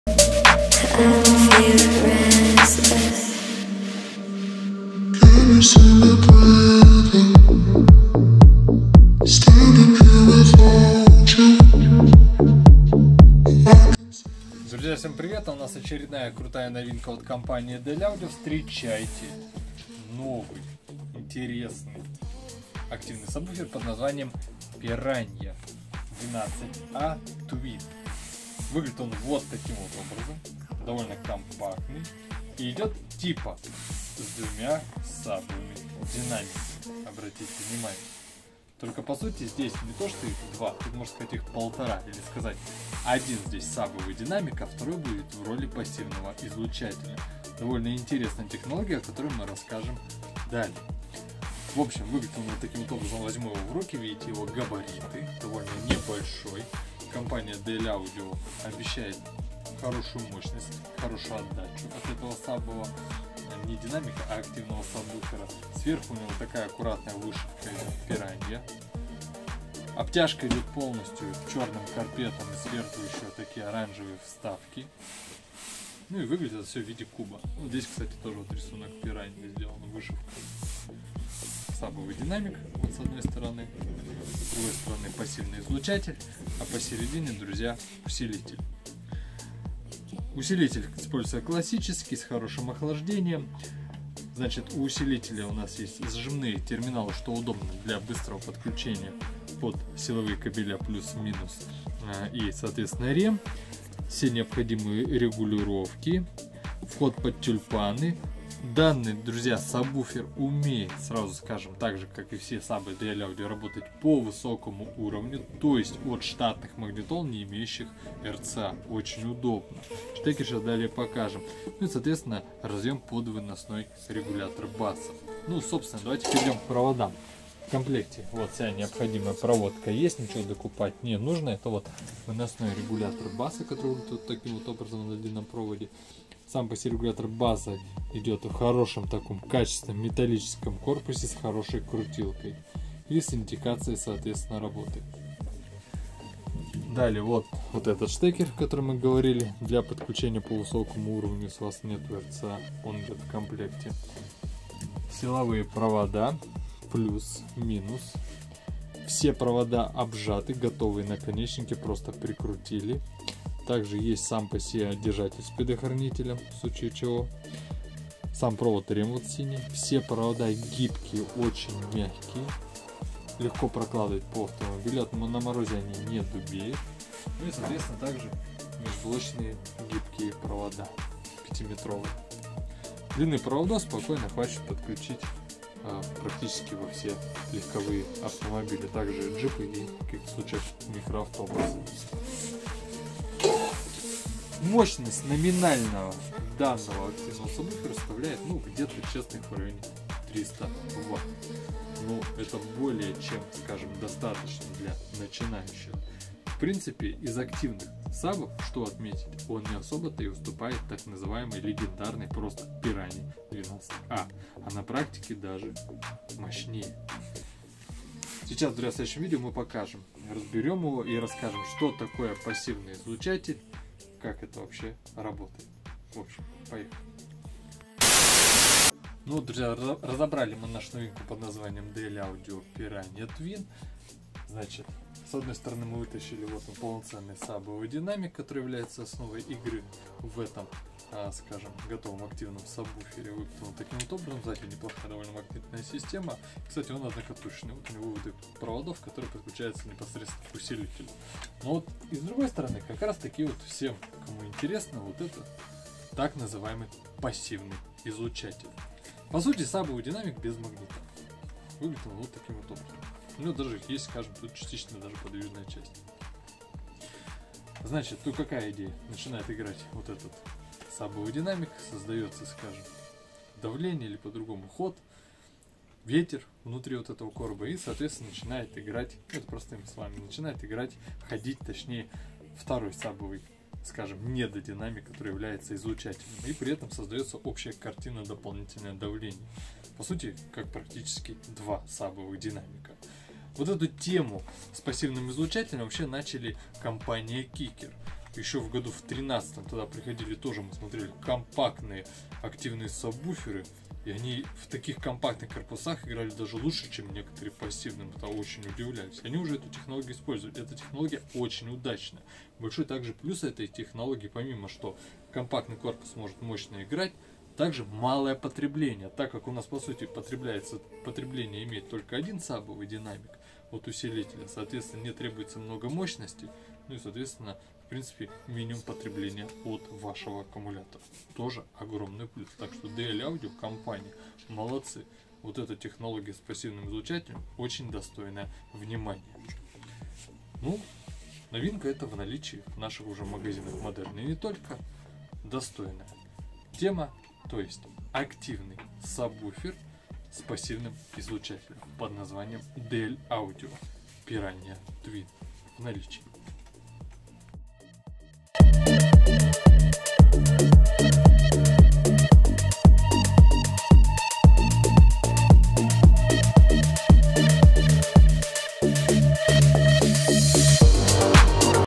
Активный сабвуфер с пассивным излучателем - DL Audio Piranha 12A Twin!
А вот Вам и интересная очередная новинка от компании DL Audio!

Такую технологию как саб с пассивным излучателем мы встречали аж в далеком 2013 году от компании Kicker! И тогда мы были удивлены хорошим результатом баса! Dl Audio Piranha 12A  это уверенное продолжение такой технологии и тоже с весьма хорошим результатом!